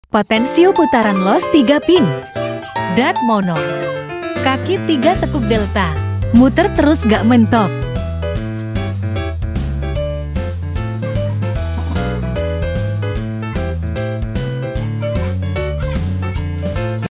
Potensio Putaran Loss 3 pin Drat Mono Kaki 3 Tekuk Delta
*HARAP PILIH PACKING BUBBLE untuk pengiriman yang lebih aman, packing standard pakai plastik*

Potensio Putaran Loss 3 pin Drat

Muter terus ga mentok

Dimensi body Keseluruhan:
Tinggi: 31mm
Lebar: 16mm
Panjang: 19mm

Diameter Drat: 9mm
Tinggi drat: 6mm

Tiang plastik full
Tinggi: 13mm
Diameter: 6mm

Kaki 3 pin tekuk
Jarak antara kaki 5mm

Sudah termasuk mur dan ring

Tersedia KNOP untuk potensio putar, kaka bisa sekalian order atau kalo kaka cari potensio model lain juga bisa tanya tanya kami di chat ya..